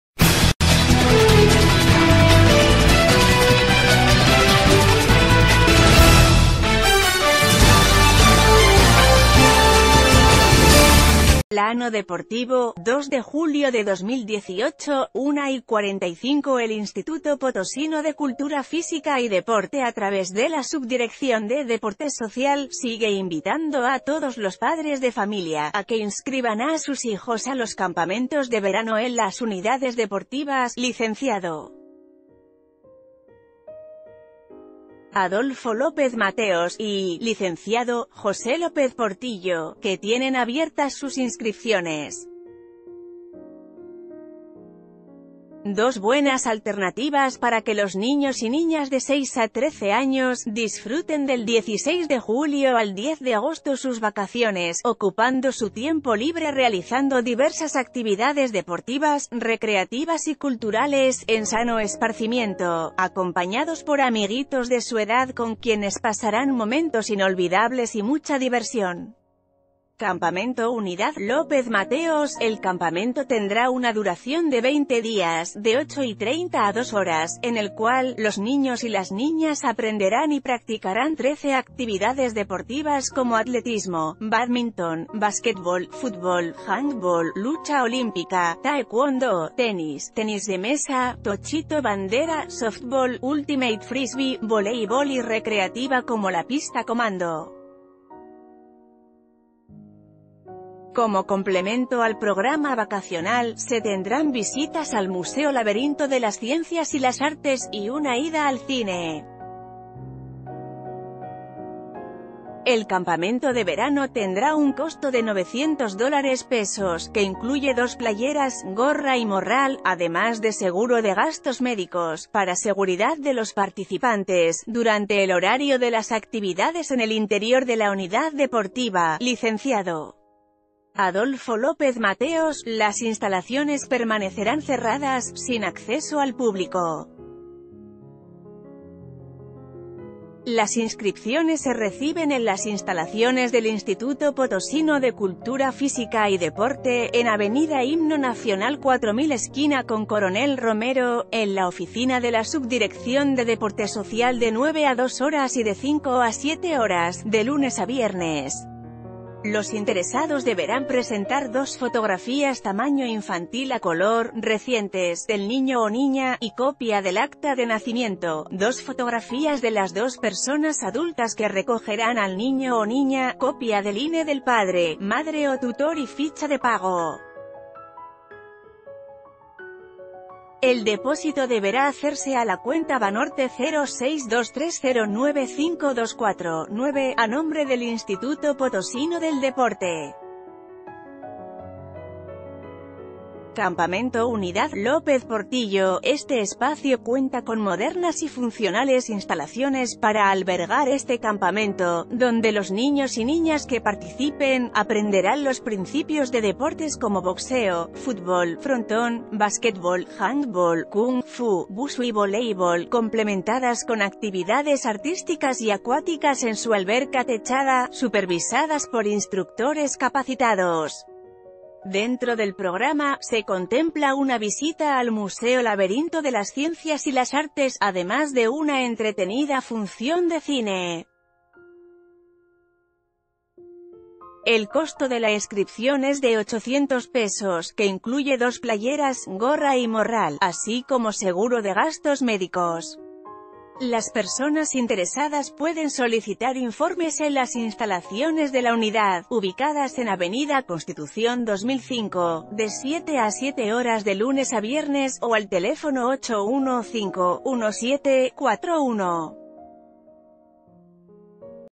INPODE invita a sus campamentos de Verano
Plano Deportivo | 02 Julio 2018 | 13:45 El Instituto Potosino de Cultura Física y Deporte a través de la Subdirección de Deporte Social, sigue invitando a todos los padres de familia, a que inscriban a sus hijos a los campamentos de Verano en las Unidades Deportivas “Lic. Adolfo López Mateos” y “Lic...